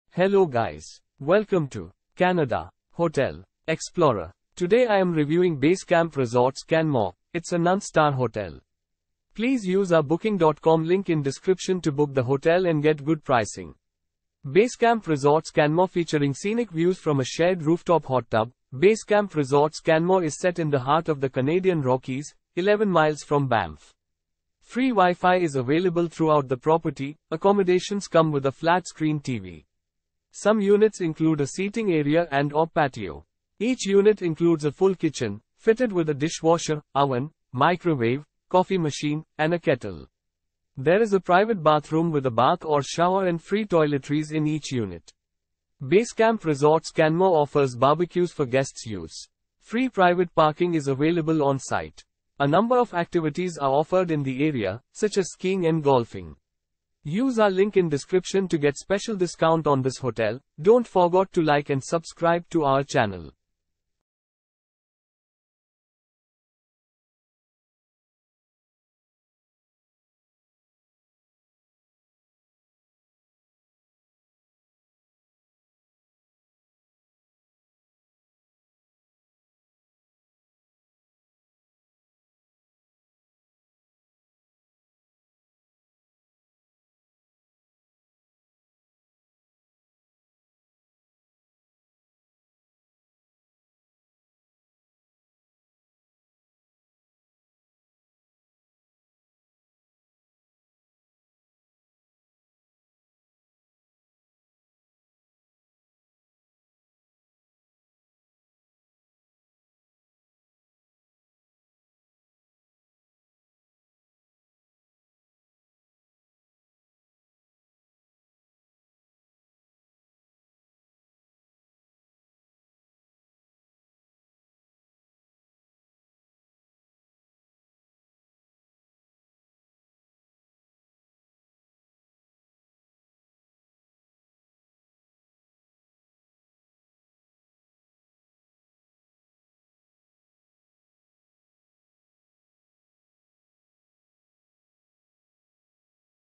Basecamp Resorts Canmore Review - Canmore , Canada
Basecamp Resorts Canmore

 Basecamp Resorts Canmore
Featuring scenic views from a shared rooftop hot tub, Basecamp Resorts Canmore is set in the heart of the Canadian Rockies, 11 miles from Banff. Free WiFi is available throughout the property,

Accommodations come with a flat-screen TV. Some units include a seating area and/or patio. Each unit includes a full kitchen, fitted with a dishwasher, oven, microwave, coffee machine, and a kettle. There is a private bathroom with a bath or shower and free toiletries in each unit.

Basecamp Resorts Canmore offers barbecues for guests' use. Free private parking is available on site. A number of activities are offered in the area, such as skiing and golfing.